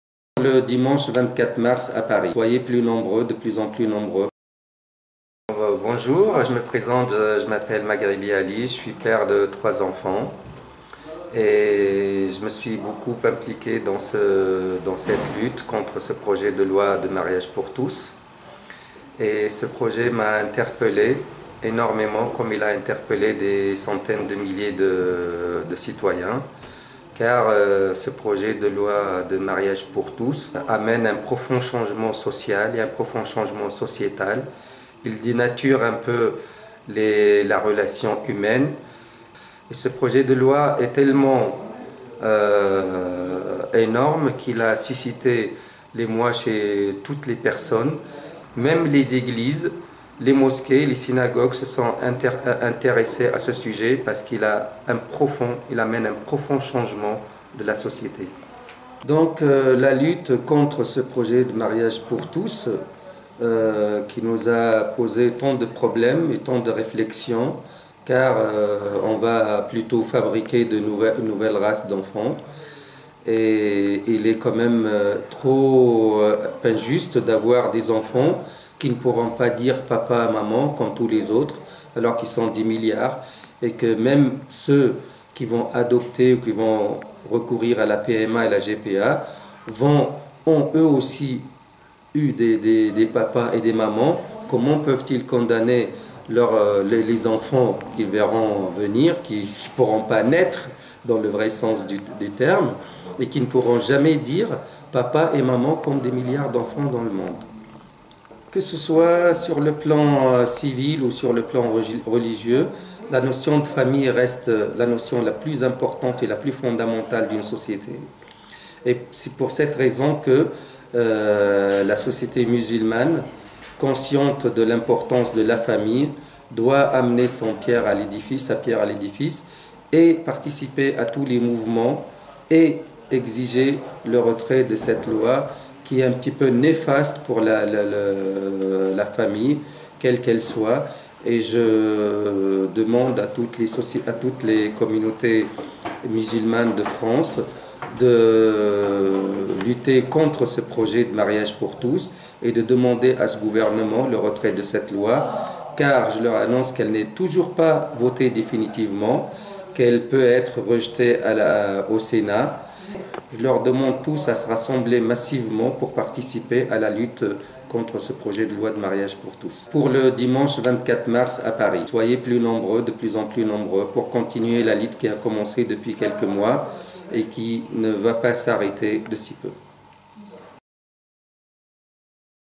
Ali _Musulmans : dimanche 24 mars à Paris, soyez nombreux, de plus en plus nombreux !
Vidéo publiée le 1er mars 2013 par Musulmans Pour l'Enfance.
Musulmans : dimanche 24 mars à Paris, soyez nombreux, de plus en plus nombreux  !
" Je m'appelle M. Ali, je suis père de trois enfants. Je me suis beaucoup impliqué dans cette lutte contre ce projet de loi du mariage pour tous. Ce projet de loi m'a interpellé énormément, comme il a interpellé des centaines de milliers de citoyens. Car ce projet de loi du mariage pour tous amène un profond changement social et un profond changement sociétal, il dénature un peu la relation humaine. Ce projet de loi est tellement énorme qu'il a suscité l'émoi chez toutes les personnes. Même les églises, les mosquées, les synagogues se sont intéressées à ce sujet, parce qu'il amène un profond changement de la société. Donc, la lutte contre ce projet de mariage pour tous, qui pose tant de problèmes, tant de réflexions, car on va fabriquer de nouvelles races d'enfants - Il est quand même trop injuste d'avoir des enfants qui ne pourront pas dire "papa, maman" quand tous les autres, alors qu'ils sont des milliards, alors que même ceux qui vont adopter, qui vont recourir à la PMA et la GPA ont, eux aussi, eu des papas et des mamans, comment peuvent-ils condamner les enfants qui vont venir chez eux, qui ne vont pas naître dans le vrai sens du terme ? et qui ne pourront jamais dire "papa" ou "maman" comme des milliards d'enfants dans le monde ? Que ce soit sur le plan civil ou sur le plan religieux, la notion de famille reste la notion la plus importante et la plus fondamentale d'une société ! Et c'est pour cette raison que la société musulmane, consciente de  l'importance de la famille, doit amener sa pierre à l'édifice, participer à tous les mouvements et exiger le retrait de cette loi qui est néfaste pour la famille, quelle qu'elle soit. Et je demande à toutes les communautés musulmanes de France de lutter contre ce projet de projet de mariage pour tous et de demander à ce gouvernement le retrait de cette loi. Car je leur annonce qu'elle n'est toujours pas votée définitivement, elle peut être rejetée au Sénat. Je leur demande tous à se rassembler massivement pour participer à la lutte contre ce projet de loi du mariage pour tous. Le dimanche 24 mars à Paris, soyez nombreux, de plus en plus nombreux, pour continuer la lutte qui a commencé il y a quelques mois et qui ne va pas s'arrêter de sitôt ! C'est une ouverture à tous les droits, sans limite ! "